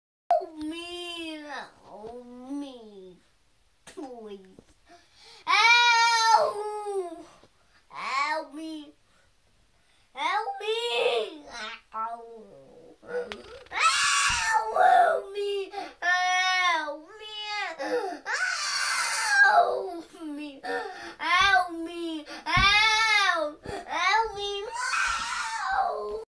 Kids Drama ll iPad Tantrums during Coronavirus home stay
Keeping kids happy